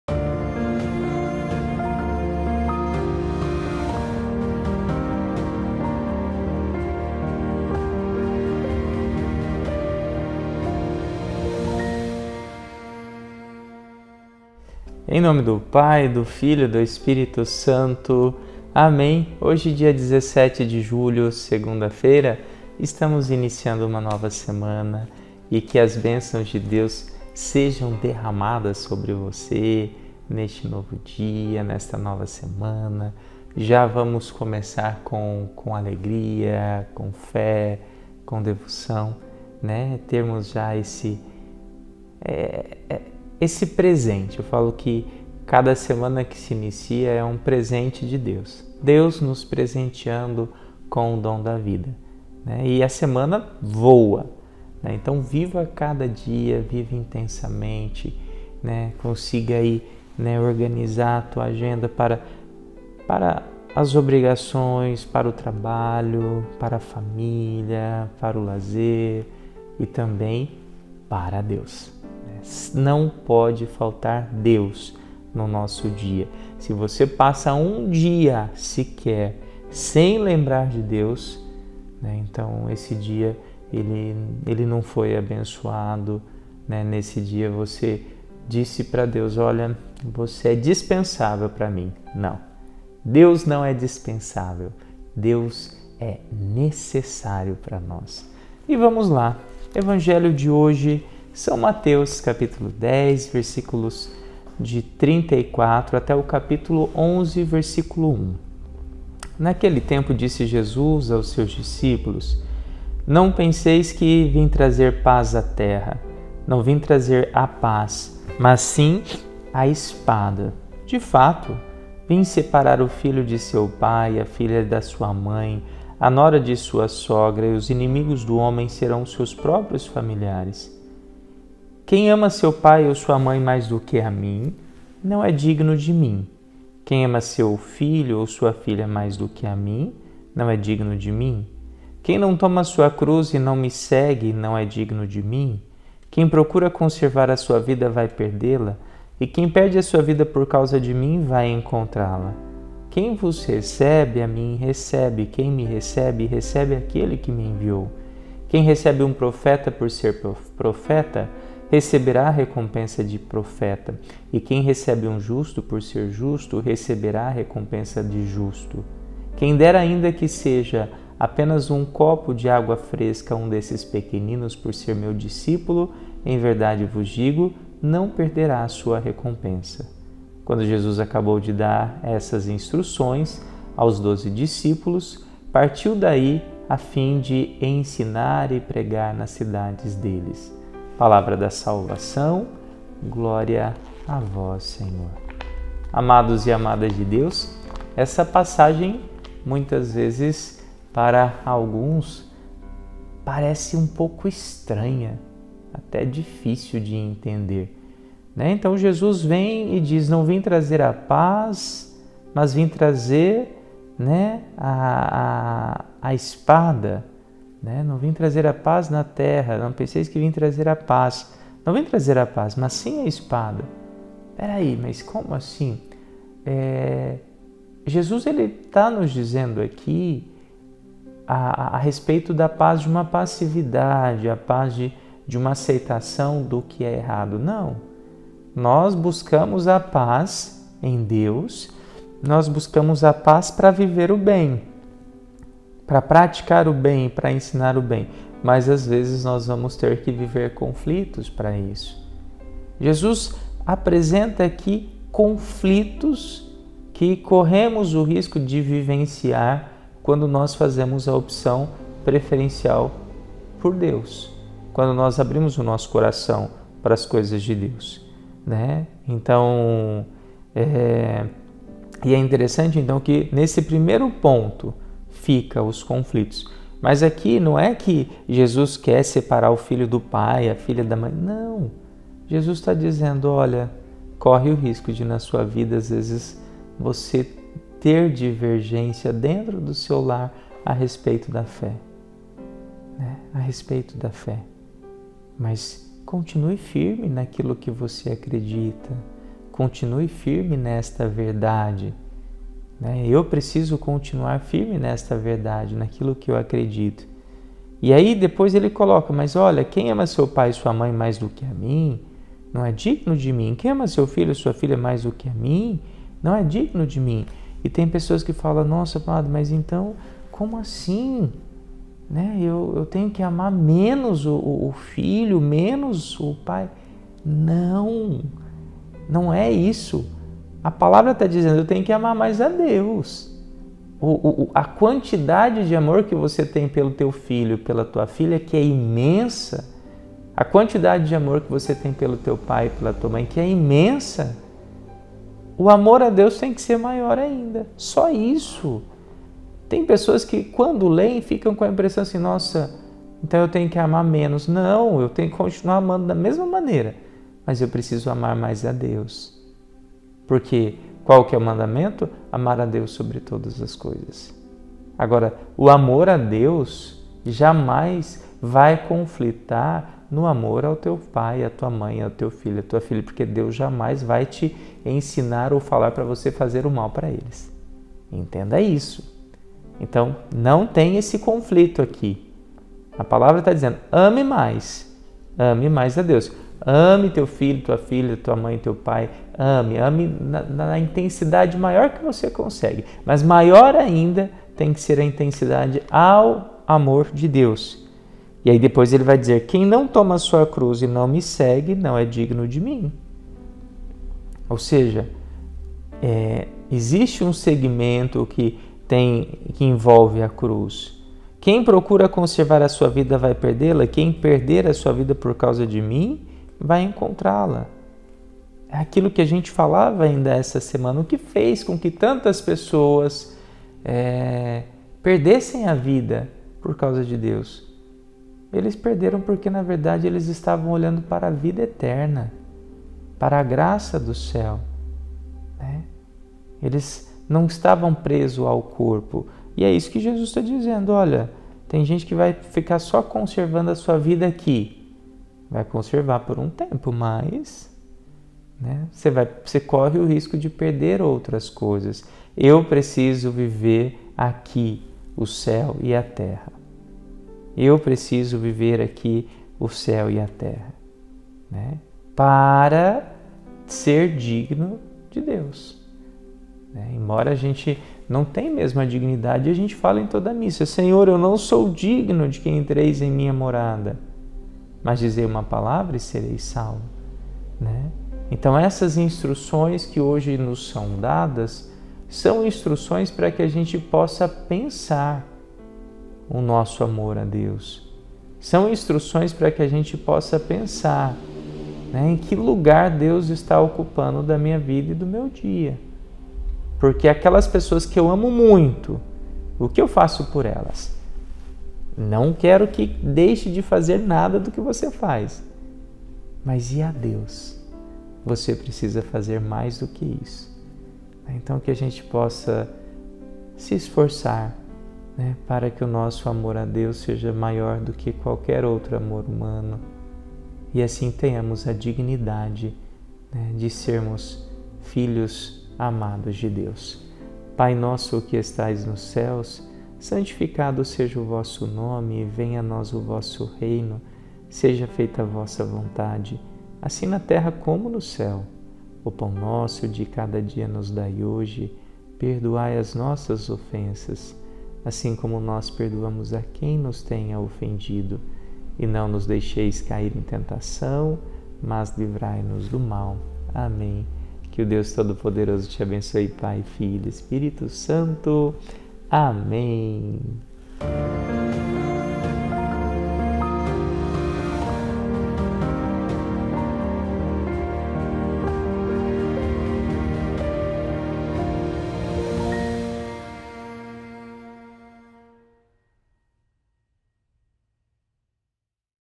Homilia Diária - Segunda-Feira - Beato Inácio de Azevedo
O cultivo do medo geralmente converge para a quebra da fraternidade, violência e injustiça, O seguimento de Jesus pressupõe dura luta, porque implica renunciar aos laços que são obstáculo para edificação do Reino.